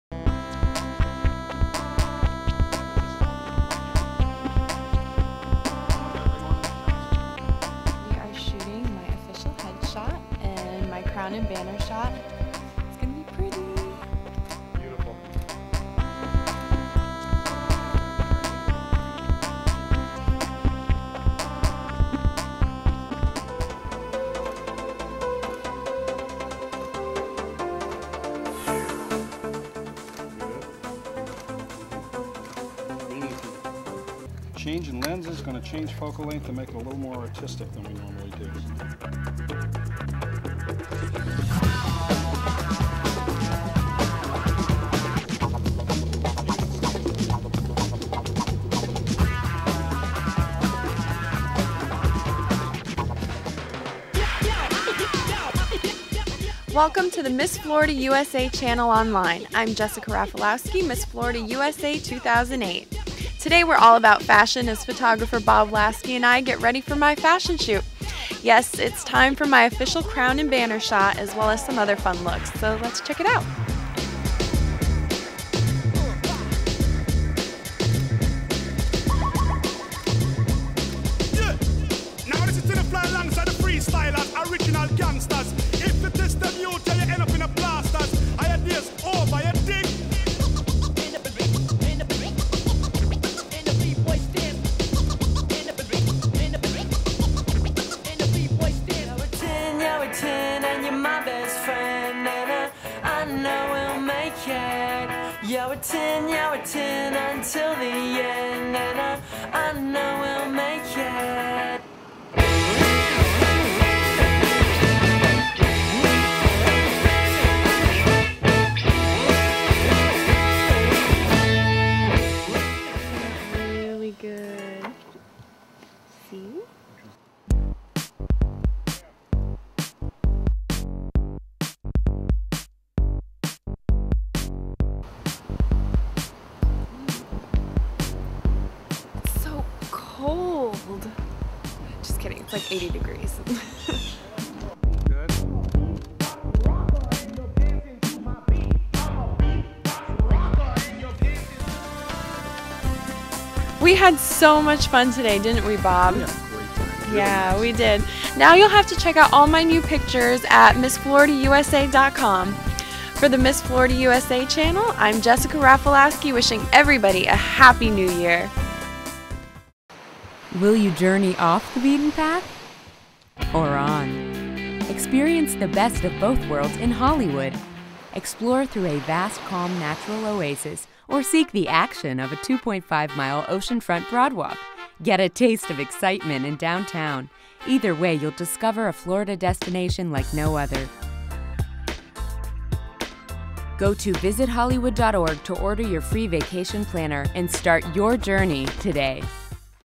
Florida USA Photo Shoot w. Bob Lasky
A highlight every year is the official photo session with Bob Lasky of Photos By Lasky. He has shot for all of the big magazines over his illustrious career and always makes time for Miss Florida USA! Enjoy the shoot.